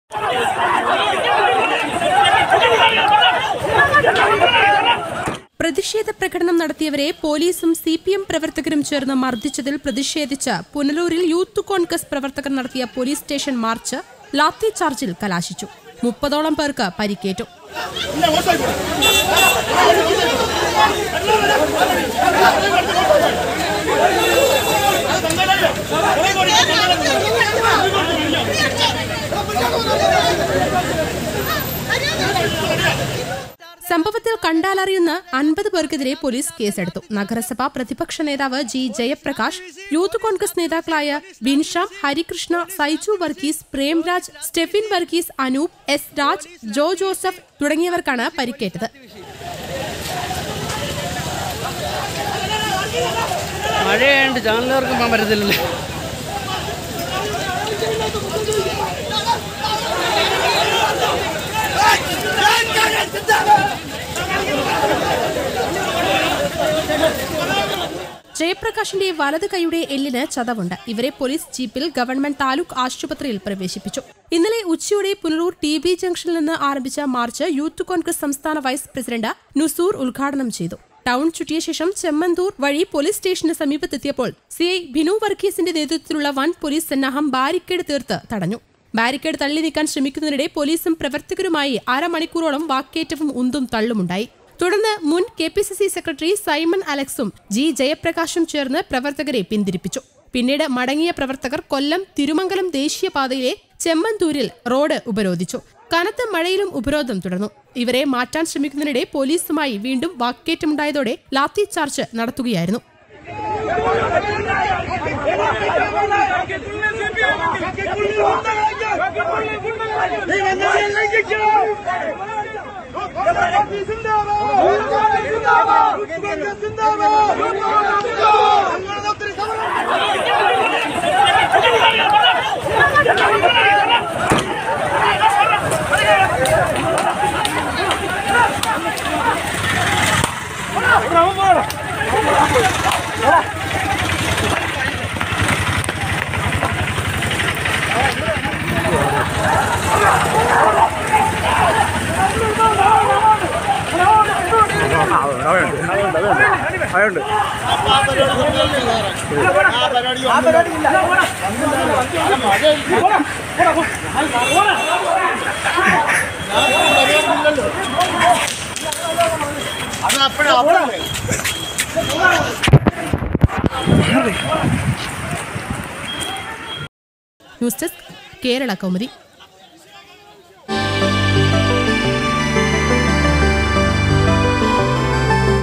കൊല്ലം പുനലൂരിൽ ലാത്തിച്ചാർജ്ജ്, 30 യൂത്ത് കോൺഗ്രസ് പ്രവർത്തകർക്ക് പരിക്ക്, 50 പേർക്കെതിരെ കേസ്
പ്രതിഷേധ പ്രകടനം നടത്തിയവരെ പൊലീസും സി.പി.എം പ്രവർത്തകരും ചേർന്ന് മർദ്ദിച്ചതിൽ പ്രതിഷേധിച്ച് പുനലൂരിൽ യൂത്ത് കോൺഗ്രസ് പ്രവർത്തകർ നടത്തിയ പൊലീസ് സ്റ്റേഷൻ മാർച്ച് ലാത്തിച്ചാർജ്ജിൽ കലാശിച്ചു. 30 ഓളം പേർക്ക് പരിക്കേറ്റു. സംഭവത്തിൽ കണ്ടാലറിയാവുന്ന 50 പേർക്കെതിരെ പൊലീസ് കേസെടുത്തു. നഗരസഭ പ്രതിപക്ഷ നേതാവ് ജി.ജയപ്രകാശ്, യൂത്ത് കോൺഗ്രസ് നേതാക്കളായ ബിൻഷാം, ഹരികൃഷ്ണ, സൈജുവർഗീസ്, പ്രേംരാജ്, െ്രസ്രഫിൻ വർഗീസ്, അനൂപ്.എസ്.രാജ്, ജോ ജോജോസഫ് തുടങ്ങിയവർക്കാണ് പരിക്കേറ്റത്. ജയപ്രകാശിന്റെ വലത് കൈയുടെ എല്ലിന് ചതവുണ്ട്. ഇവരെ പൊലീസ് ജീപ്പിൽ ഗവ. താലൂക്ക് ആശുപത്രിയിൽ പ്രവേശിപ്പിച്ചു.ഇന്നലെ ഉച്ചയോടെ പുനലൂർ ടി.ബി ജംഗ്ഷനിൽ നിന്ന് ആരംഭിച്ച മാർച്ച് യൂത്ത്‌കോൺഗ്രസ് സംസ്ഥാന വൈസ് പ്രസിഡന്റ് നുസൂർ ഉദ്ഘാടനം ചെയ്തു. ടൗൺ ചുറ്റിയ ശേഷം ചെമ്മന്തൂർ വഴി പൊലീസ് സ്റ്റേഷന് സമീപമെത്തിയപ്പോൾ സി.ഐ ബിനു വർഗീസിന്റെ നേതൃത്വത്തിലുള്ള വൻ പൊലീസ് സന്നാഹം ബാരിക്കേട് തീർത്ത് തടഞ്ഞു. ബാരിക്കേട് തള്ളി നീക്കാൻ ശ്രമിക്കുന്നതിനിടെ പൊലീസും പ്രവർത്തകരുമായി അര മണിക്കൂറോളം വാക്കേറ്റവും ഉന്തും തള്ളുമുണ്ടായി.തുടർന്ന് മുൻ കെ.പി.സി.സി സെക്രട്ടറി സൈമൺ അലക്സും ജി.ജയപ്രകാശും ചേർന്ന് പ്രവർത്തകരെ പിന്തിരിപ്പിച്ചു. പിന്നീട് മടങ്ങിയ പ്രവർത്തകർ കൊല്ലംതിരുമംഗലം ദേശീയപാതയിലെ ചെമ്മന്തൂരിൽ റോഡ് ഉപരോധിച്ചു. കനത്ത മഴയിലും ഉപരോധം തുടർന്നു. ഇവരെ മാറ്റാൻ ശ്രമിക്കുന്നതിനിടെ പൊലീസുമായി വീണ്ടും വാക്കേറ്റമുണ്ടായതോടെ ലാത്തി ചാർജ്ജ് നടത്തുകയായിരുന്നു.